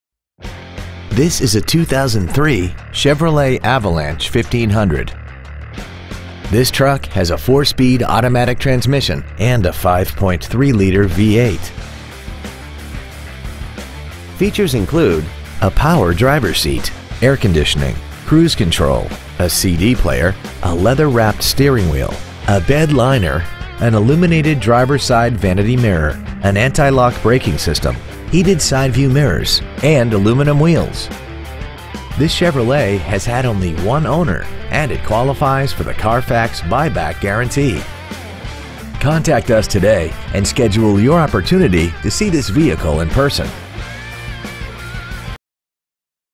Used 2003 Chevrolet Avalanche Corpus Christi TX 78412
Year : 2003
 Make : Chevrolet
 Model : Avalanche
 Engine : Gas V8 5.3L/327
 Trans . : Automatic
 Exterior : Black
 Miles : 128,778
 Interior : Gray

 6650 South Padre Island Drive